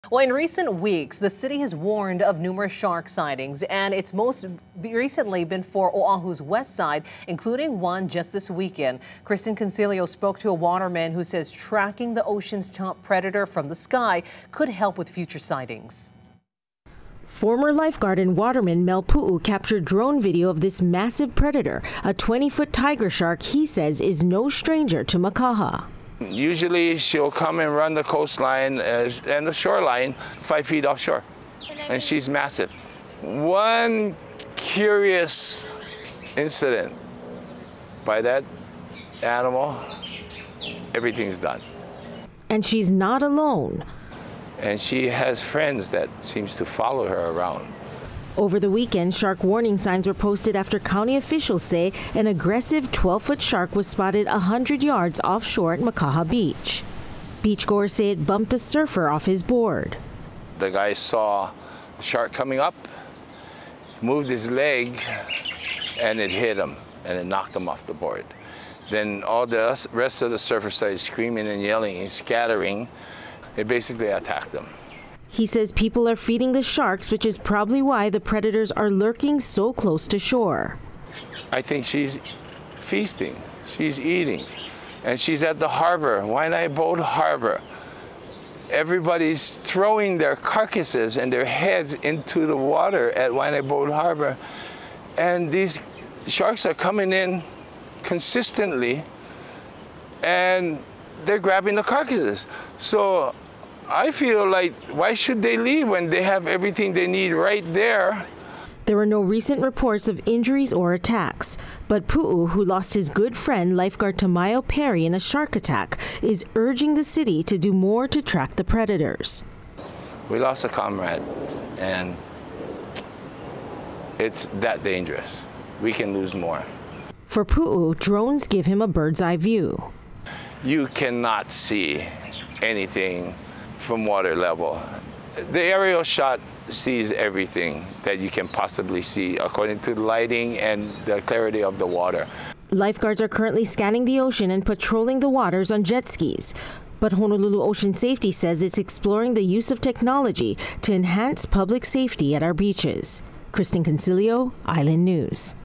How can drone cameras be used for shark sightings on Oahu?
Many shark sightings have been reported over the past several weeks, including a 20-foot tiger shark caught on video, which is why lifeguards want the city to do more about these ocean predators.